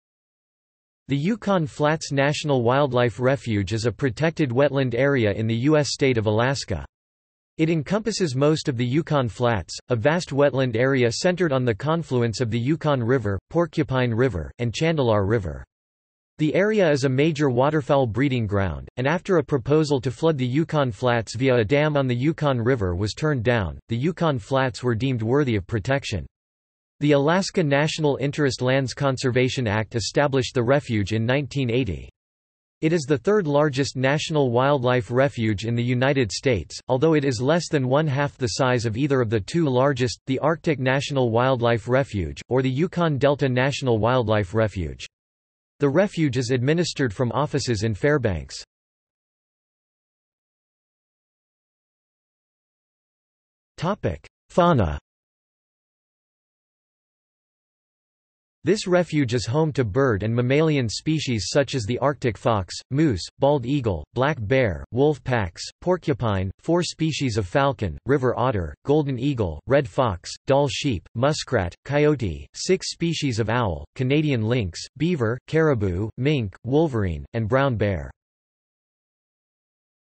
Yukon Flats National Wildlife Refuge | Wikipedia audio article
This is an audio version of the Wikipedia Article:
Yukon Flats National Wildlife Refuge

00:00:50 undefined

- Socrates

SUMMARY
The Yukon Flats National Wildlife Refuge is a protected wetland area in the U.S. state of Alaska. It encompasses most of the Yukon Flats, a vast wetland area centered on the confluence of the Yukon River, Porcupine River, and Chandalar River. The area is a major waterfowl breeding ground, and after a proposal to flood the Yukon Flats via a dam on the Yukon River was turned down, the Yukon Flats were deemed worthy of protection. The Alaska National Interest Lands Conservation Act established the refuge in 1980. It is the third-largest National Wildlife Refuge in the United States, although it is less than one-half the size of either of the two largest, the Arctic National Wildlife Refuge, or the Yukon Delta National Wildlife Refuge. The refuge is administered from offices in Fairbanks.